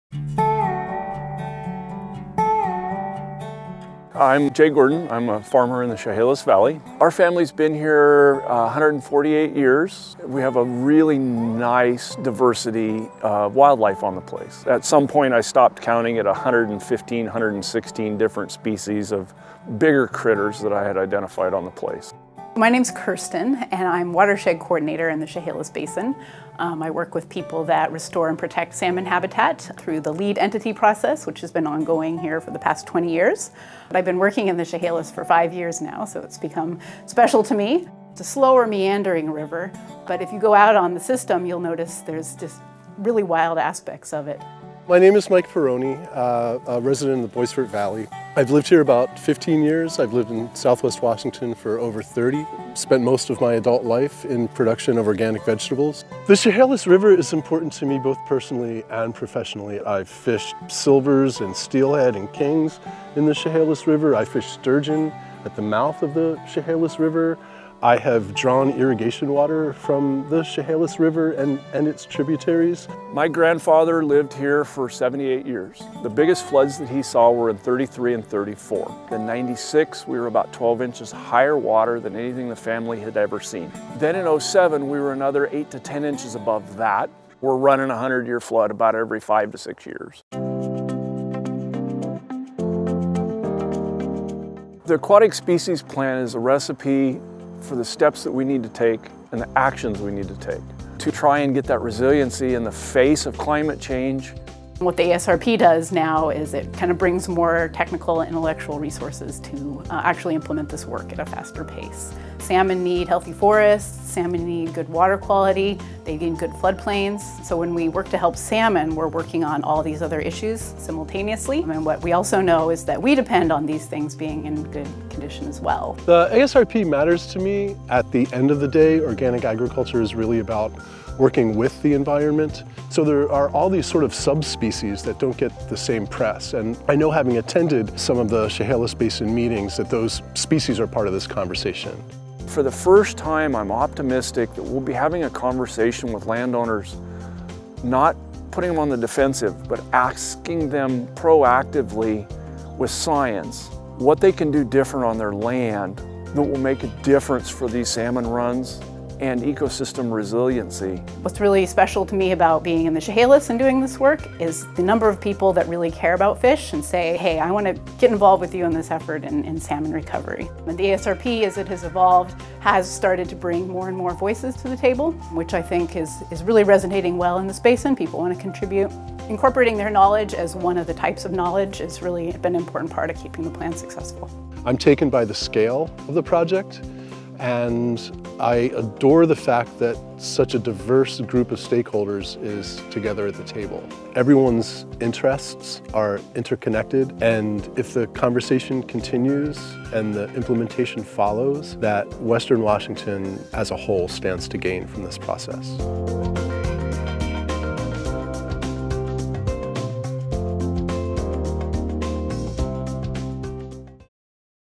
Aquatic Species Restoration Plan (ASRP) for the Chehalis Basin
To help address the downward trend of salmon runs in the Chehalis River Basin, representatives from the Quinault Indian Nation, Confederated Tribes of the Chehalis Reservation, Washington Department of Fish and Wildlife, and other technical experts developed a draft aquatic species restoration plan aimed at protecting and restoring aquatic habitat in the basin.

Together, the team worked with farmers, foresters, conservationists, the Department of Ecology’s Office of Chehalis Basin, other state agencies, and local governments to help shape the plan.

The science-based draft restoration plan identifies potential actions with the voluntary support of willing landowners that offer the best chance to 1.) Support healthy, harvestable salmon populations; 2.) Build robust, diverse populations of other native fish and wildlife species; and 3.) Foster productive ecosystems more resilient to climate change and human-caused stressors.

You are invited to comment on the draft restoration plan through Jan. 14, 2020. To review the plan and provide input,